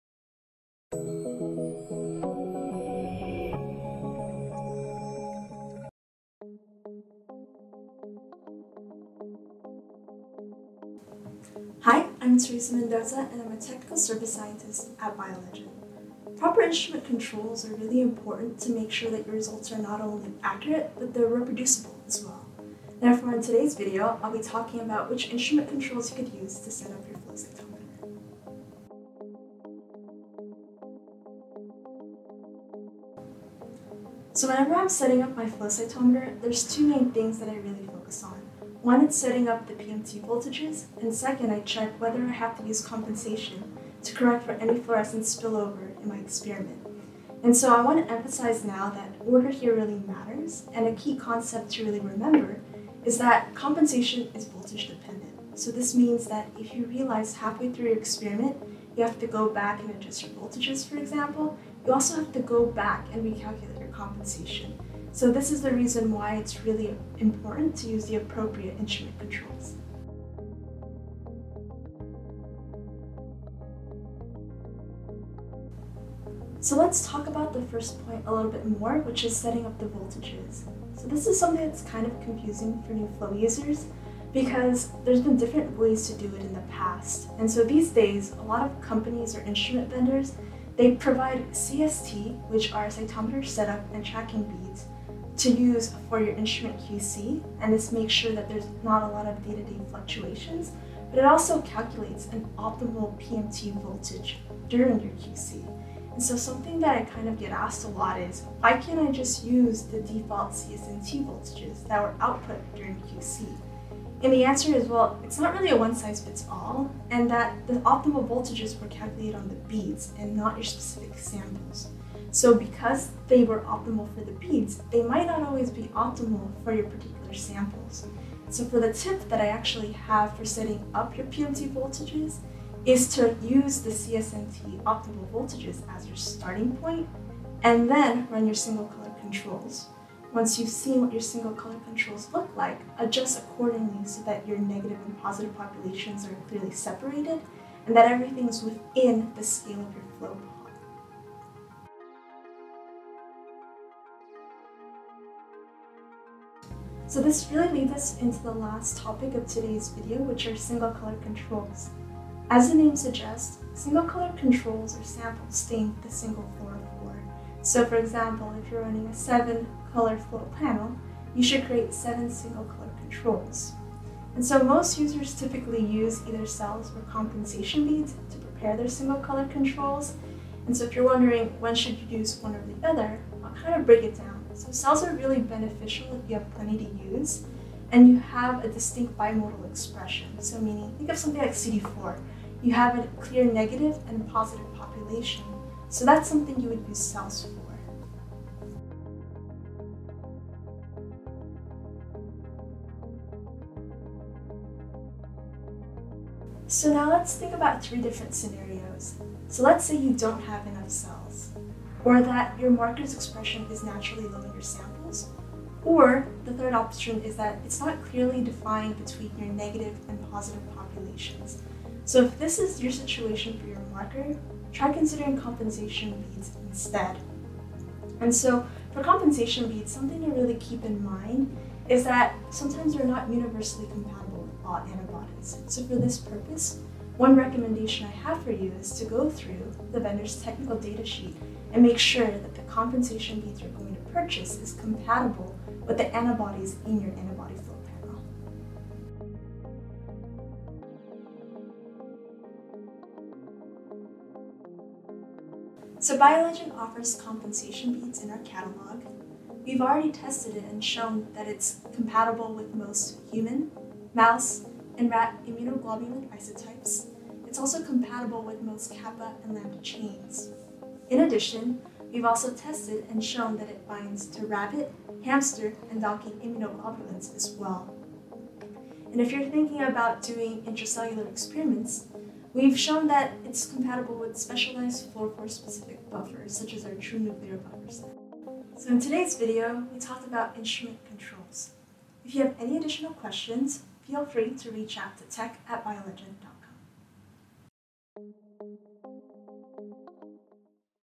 Tech Insights: Flow Cytometry Instrument Controls
Watch as Theresa Mendoza, MS, discusses setting up proper instrument controls for a flow cytometer, including compensation beads, single-color controls, and voltage settings.